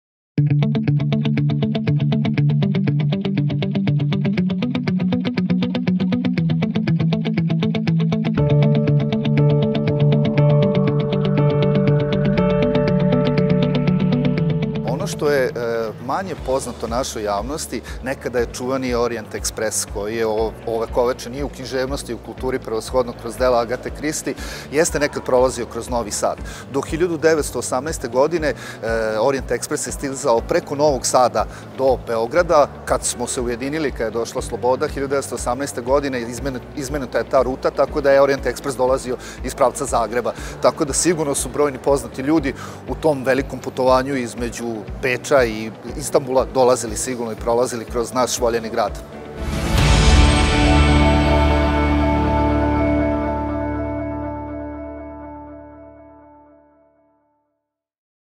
Нови Сад - Центар железничке историје / Оријент Експерс
📌 Једна од станица чувеног Оријент експреса је некада била управо у Новом Саду! Легендарни воз је саобраћао од Париза до Истанбула, био је синоним за луксузно путовање возом, а наша књижевница Исидора Секулић је говорила да је Оријент експрес “махните брзине”. Да ли знате неку занимљивост у вези са железницом у свом граду? Пишите нам у коментарима! 👇